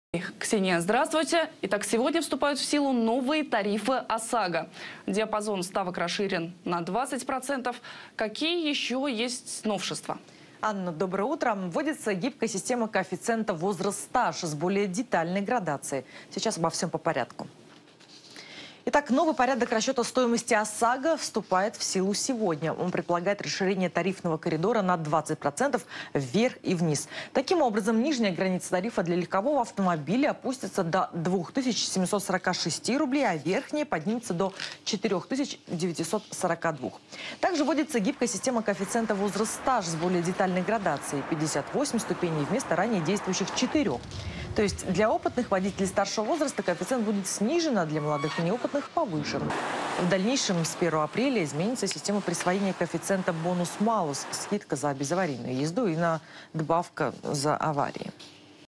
В России начали действовать новые тарифы ОСАГО
Вступает в силу новый порядок расчета стоимости обязательного автострахования (ОСАГО). Он предполагает расширение тарифного коридора на 20% вверх и вниз. Таким образом, нижняя граница тарифа для легкового автомобиля опустится до 2 746 рублей. Верхняя поднимется до 4 942 рублей.